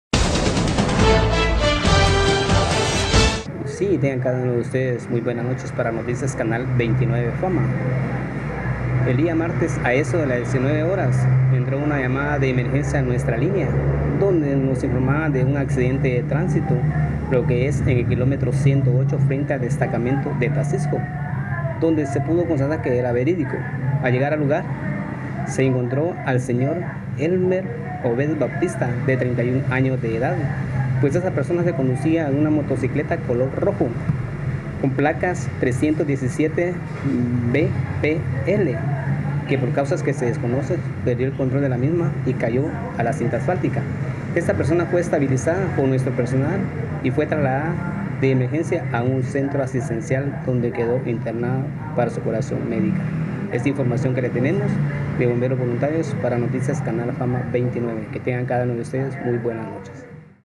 Accidente en Taxisco Santa Rosa
Bomberos Voluntarios de Chiquimulilla nos brindan informacion sobre accidente de trancito en taxisco